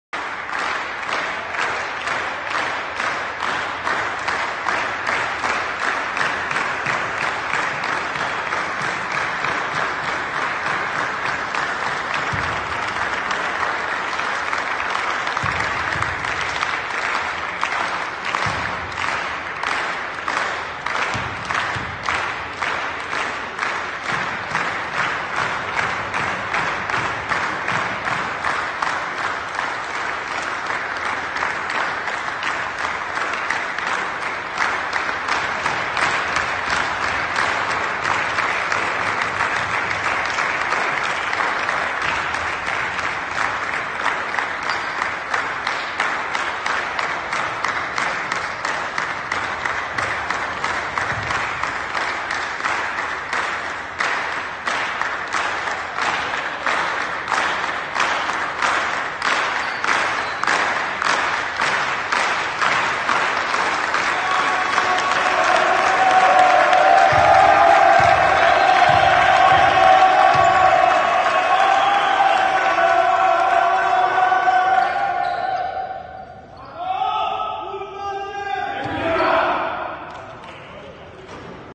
Οι παίκτες της Αργεντινής χειροκροτούν ο ένας τον άλλον
Τελετουργικό από το τέλος της προπόνησης της Αργεντινής στο Παγκόσμιο Κύπελλο 2019. Οι παίκτες χειροκροτούν ο ένας τον άλλον